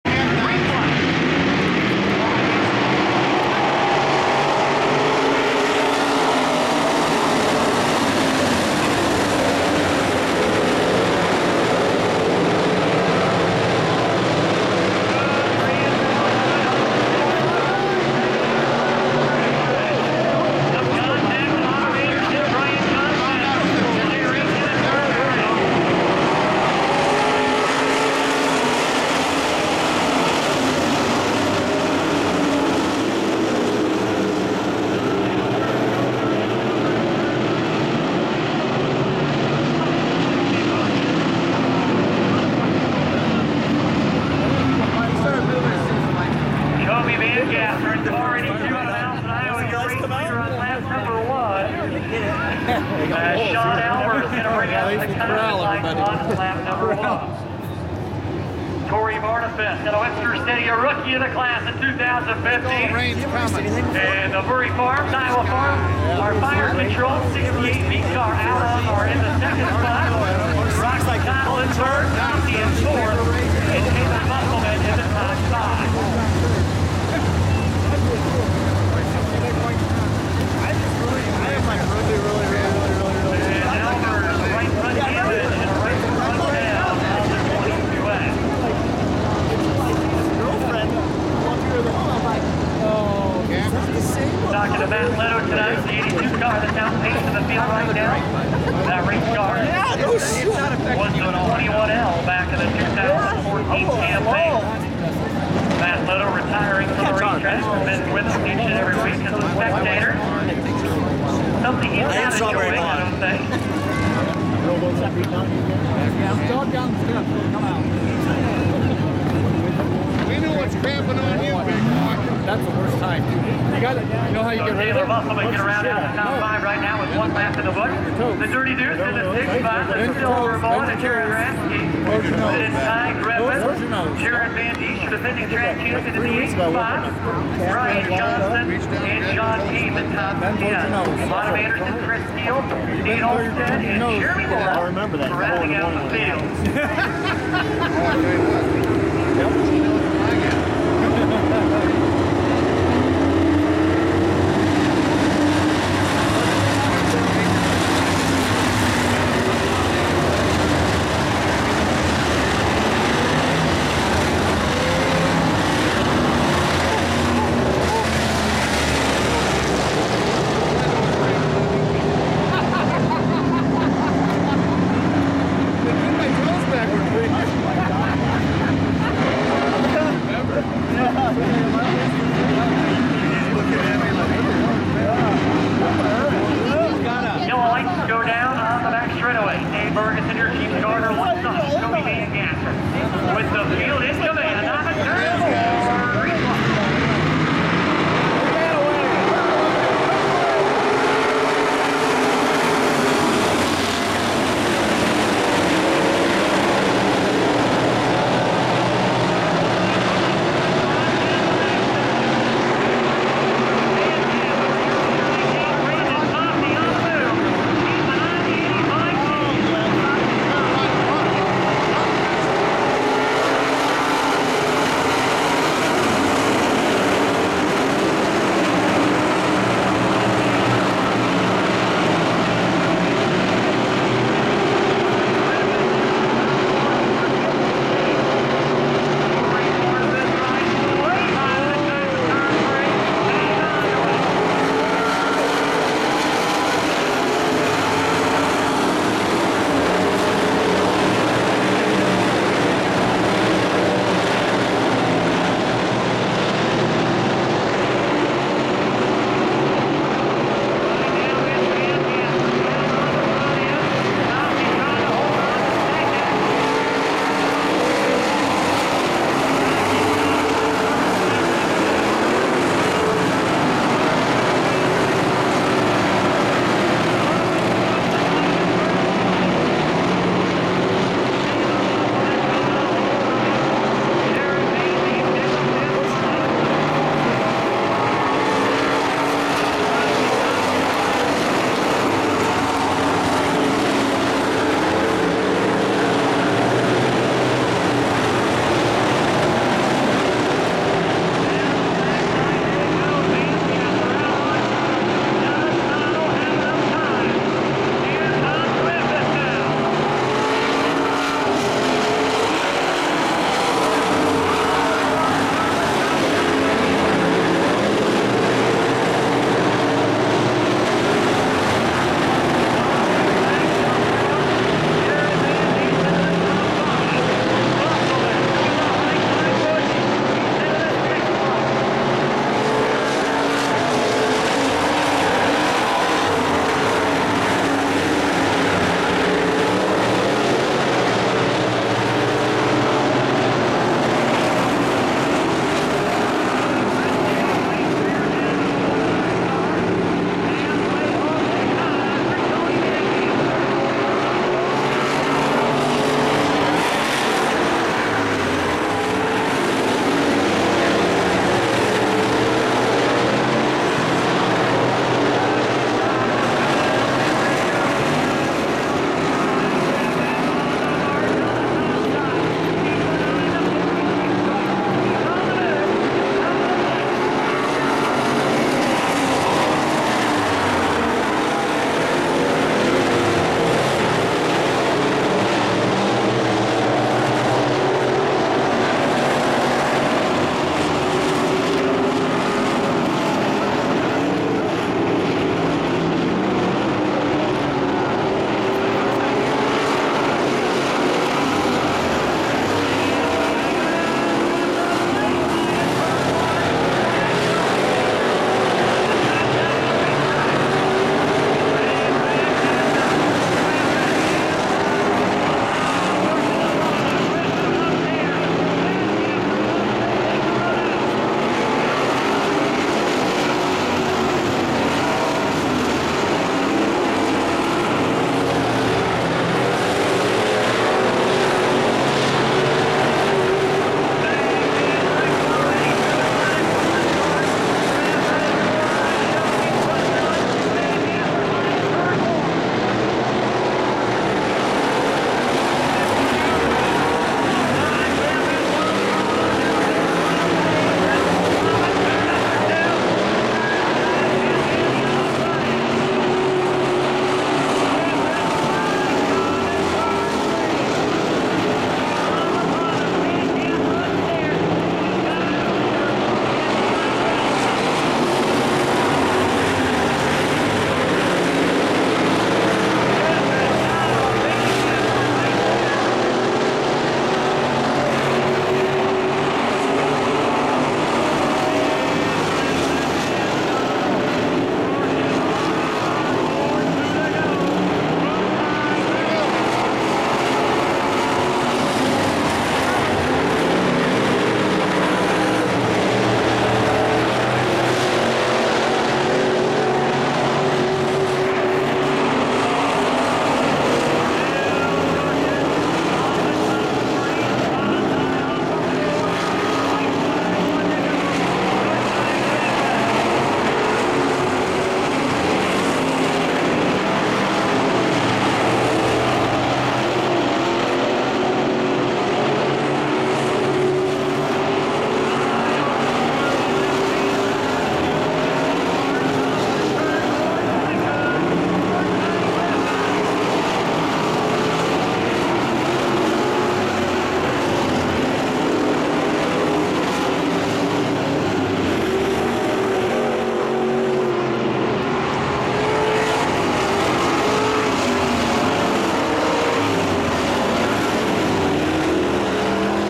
Webster City HCS Sport Mod A Main 5-23-15
56 Ty Griffith won the IMCA Sport Modified A Main.